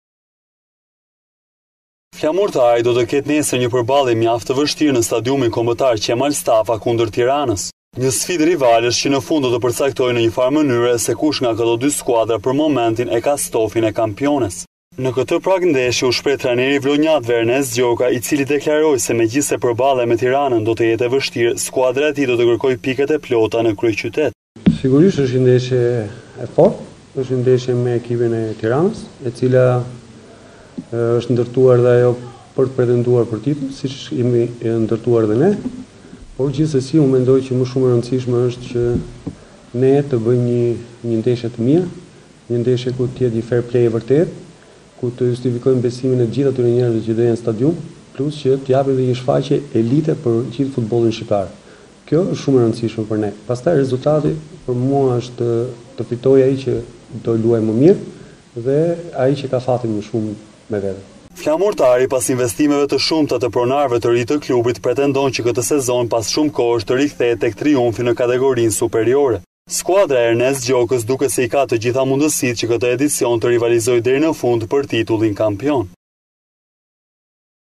Ndizet Tirana - Flamurtari. Ernest Gjoka " Të fitojë më i miri "- RTV Ora News- Lajmi i fundit-
Flamurtari do të luaj nesër supersfidën e parë të sezonit kundër Tiranës. Në prag të kësaj ndeshje u shpreh trajneri i vlonjatëve Ernest Gjoka i cili deklaroi se megjithëse përballja me bardheblutë do të jetë e vështurë skuadra e tij do të kërkoj pikët e plota në kryeyqtet

" Është një ndeshje e vështirë, por natyrisht ne do të kërkojmë më të mirën në këtë sfidë. Tirana është përforcuar gjatë verës, me lojtarë cilësorë në gjirin e vet, por edhe Flamurtari ka bëër afrime shumë të mira në skuadër, me lojtarë shumë cilësorë. Do të jetë një ndeshje e fortë , por ne duam të marrim maksimumin në këtë 90 minutësh. Skuadra ka punuar mjaft mirë gjatë gjithë javës, kampionatin e kemi nisur me një fitore përballë Teutës dhe duam të vijojmë në këtë rrugë, edhe në ndeshjen me Tiranën në Qemal Stafa. Besoj shumë tek skuadra dhe shpirti i garës në fushën e lojës dhe mendoj se do të arrijmë të dalim me sukses. " - e mbylli prononcimin e tij trajneri i vlonjatëve Ernest Gjoka

Takimi i nesërm do të japë edhe sinjalet e para se kush nga këto dy skuadra zotëron me të vërtetë stofin e kampiones.